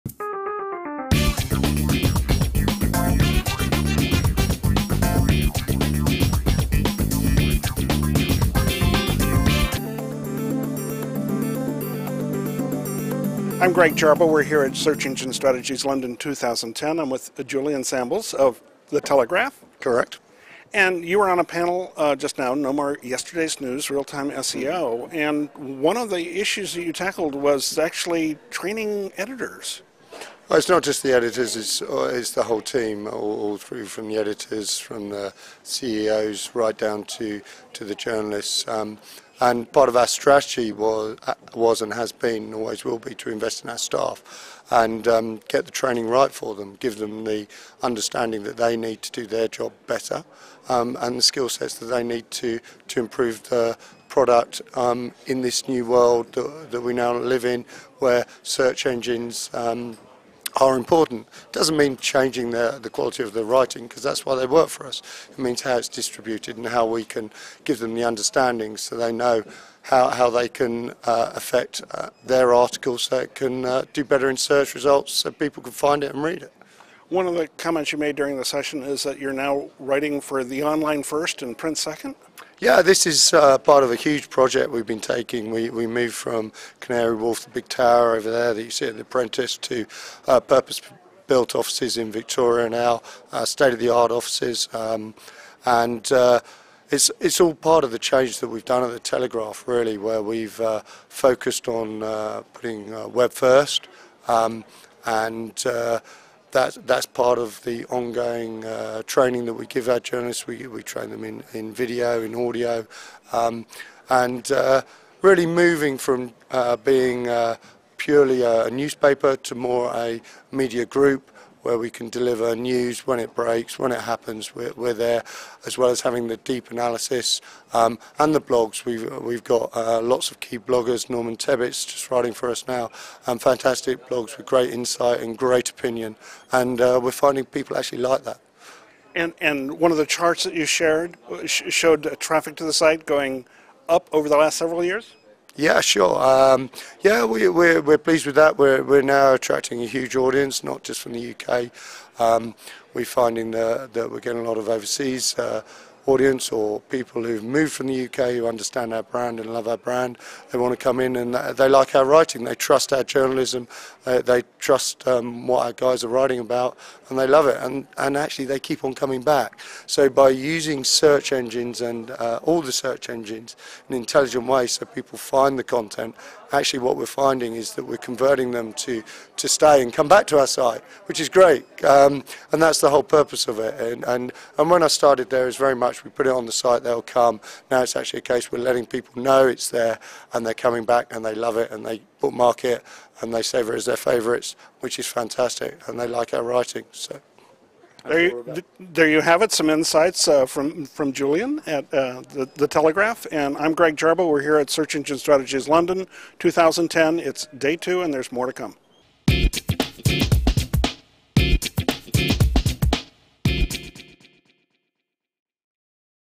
The Telegraph integrates SEO boosting online readership at SES London 2010
Greg Jarboe, SEO-PR, interviews Julian Sambles, The Telegraph, who spoke on the SES London 2010 panel, No More Yesterdays News, Real-Time SEO. Julian talks about the importance of training editors and journalists to help them understand and integrate SEO into their product. Julian says the Telegraph is now prioritizing its copy focusing on online readership first and print readership second. The Telegraph now features influential blogs as part of its online presence. As a result of incorporating SEO into its product, the Telegraph has seen its share of the online audience shoot upwards, in addition to seeing its Web traffic increase from outside of the UK. Using search engines in an intelligent way, says Julian, has enabled the Telegraph to convert new readers to its website.